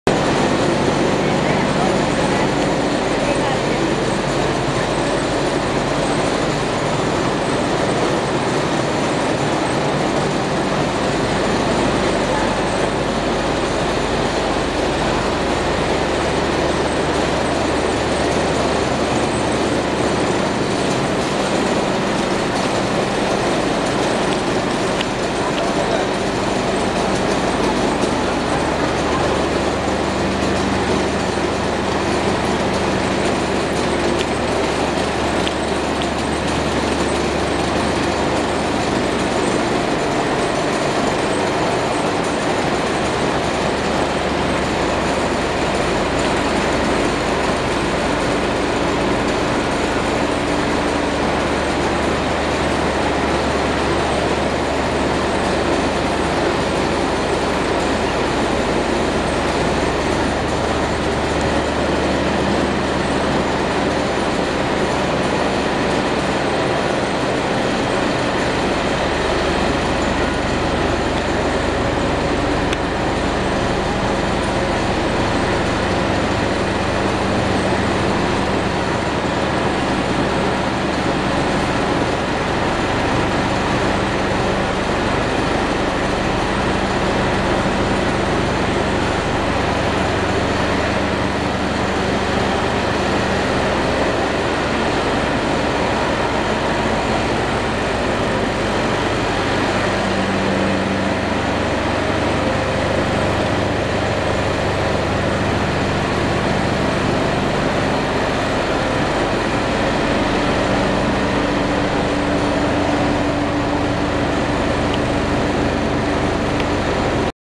From the tea gardens of Munnar to your tea pot.
See how tea is processed?